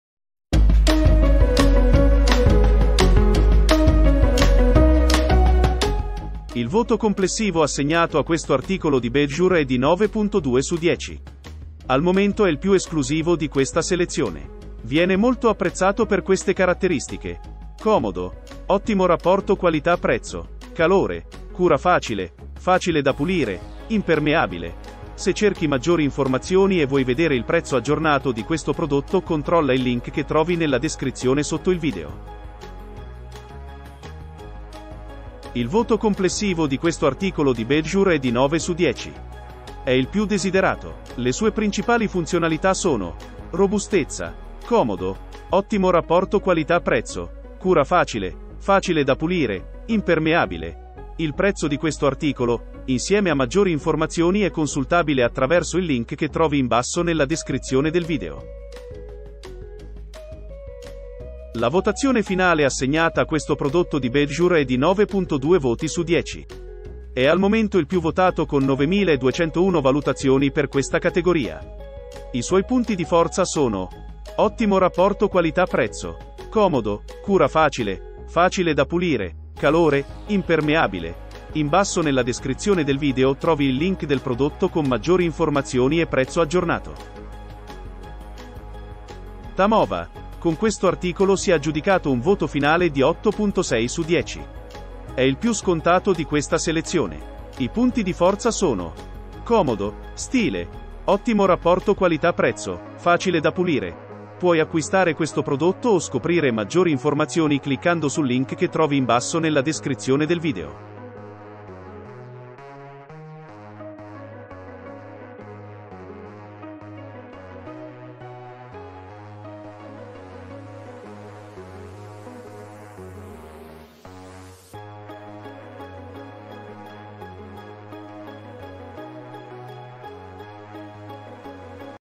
Cuscini per cani - Top 4 - (QualeScelgo)
📣 Link 'Cuscini per cani' presenti in questo video:

✅ IL PIÙ ESCLUSIVO: Bedsure Cuscino per Cane Gande - Materassino per Cani Taglia...

✅ IL PIÙ DESIDERATO: Bedsure Cuscino per Cani Taglia Grande - Cuscini per Cani Ta...

✅ IL PIÙ VOTATO: Bedsure Cuscino Cane Grande in Memory Foam - Materassino Can...

✅ IL PIÙ SCONTATO: TAMOWA Cuccia per Gatti Letto per Cani Rotondo, Peluche Ciam...

Indice:
00:00 - Intro
00:05 - Bedsure Cuscino per Cane Gande - Materassino per Cani Taglia...
00:34 - Bedsure Cuscino per Cani Taglia Grande - Cuscini per Cani Ta...
01:00 - Bedsure Cuscino Cane Grande in Memory Foam - Materassino Can...
01:28 - TAMOWA Cuccia per Gatti Letto per Cani Rotondo, Peluche Ciam...